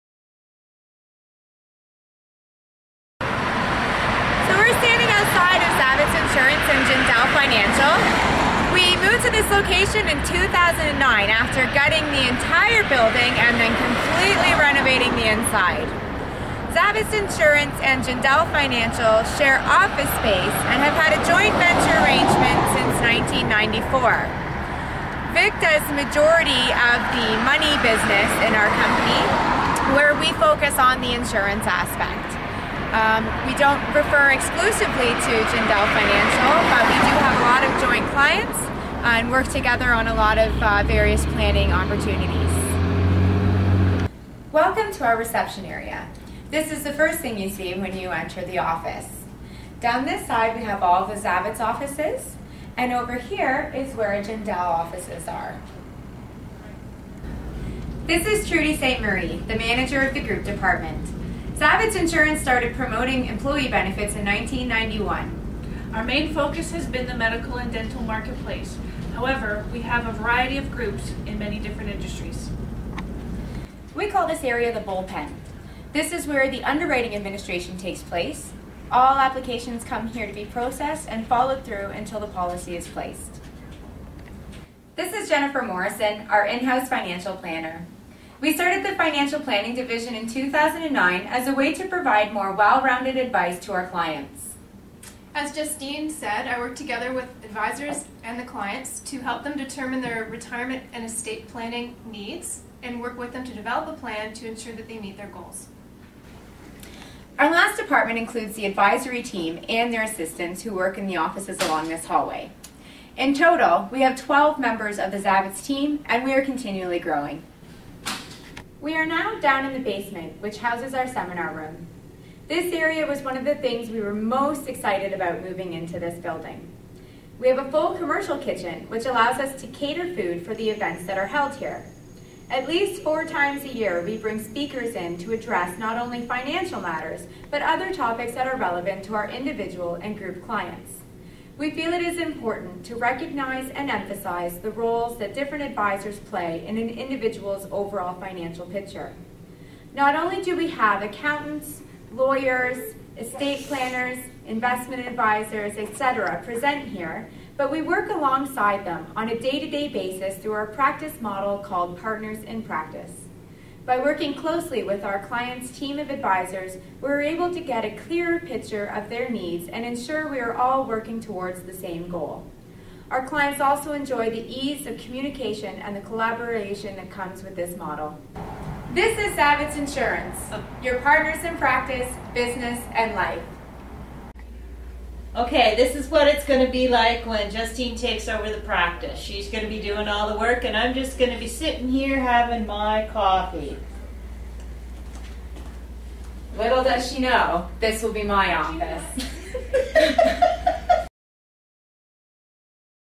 The Office of Zavitz Insurance
A tour of our office and how we operate.